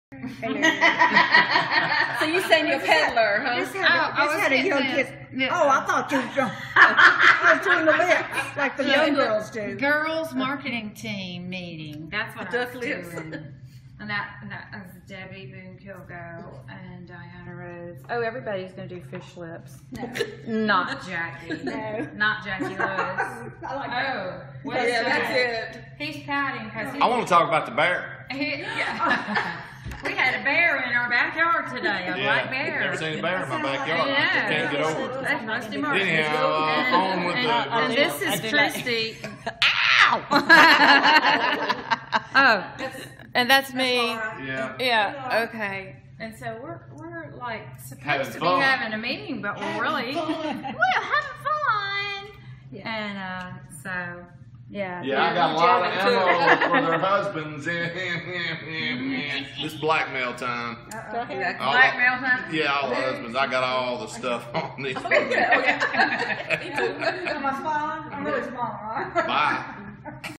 GMT Meeting #2
GMT (girls marketing team) gathers for planning meeting #2. We meet to share ideas and tasks to help move Russell L. Martin’s novel, “Scars of My Guardian Angel”. Seems like there is more laughter than actual planning!